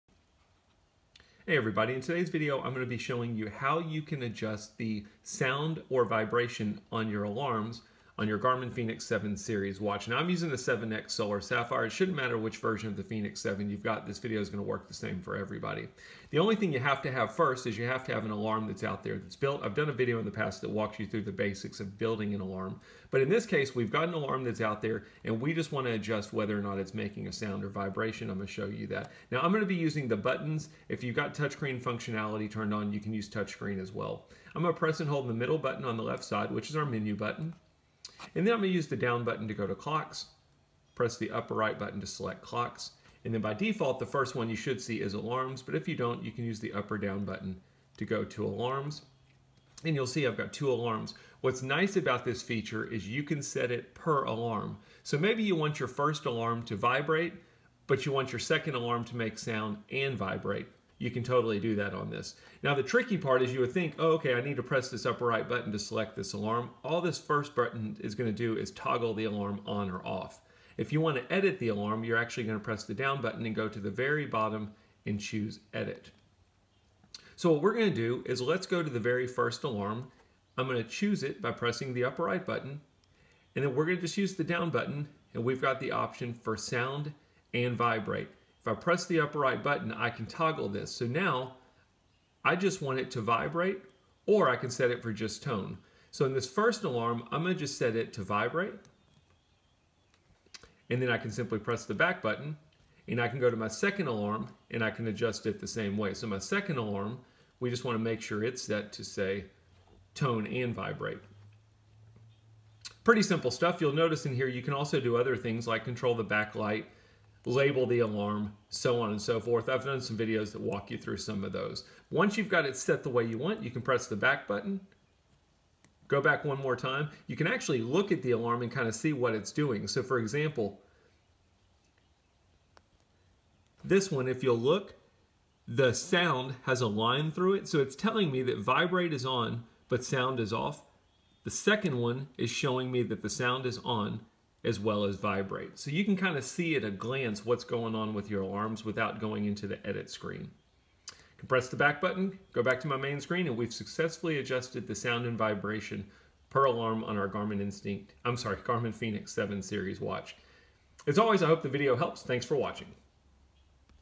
Garmin Fenix 7X: Set tone or vibrate per alarm
Garmin Fenix 7X: How to set the tone (beep) and/or vibrate per alarm